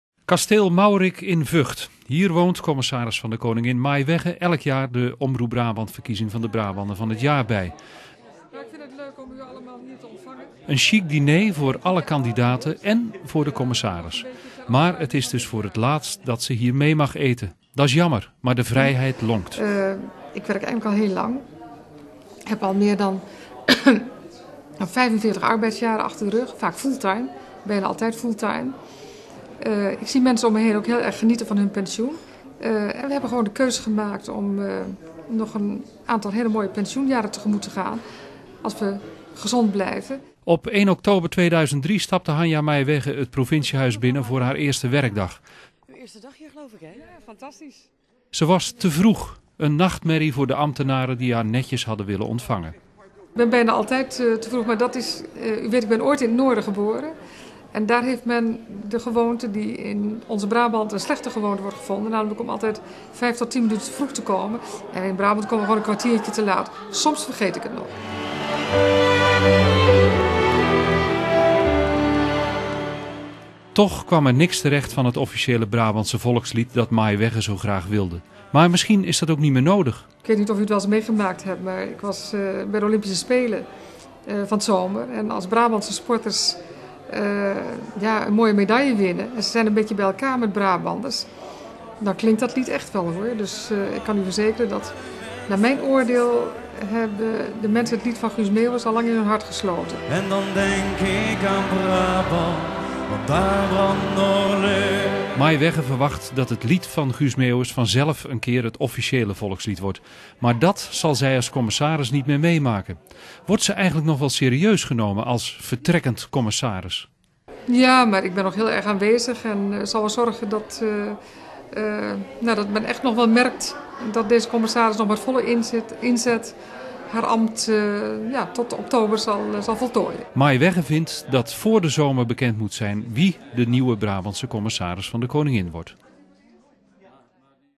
Laatste kerst Maij-Weggen als Commissaris van de Koningin
Volgend jaar rond kerst kan Hanja Maij-Weggen tijdens de feestdagen lekker bij de kerstboom blijven zitten. Want dan is ze geen Commissaris van de Koningin meer en hoeft ze dus ook geen verplichte bezoekjes af te leggen.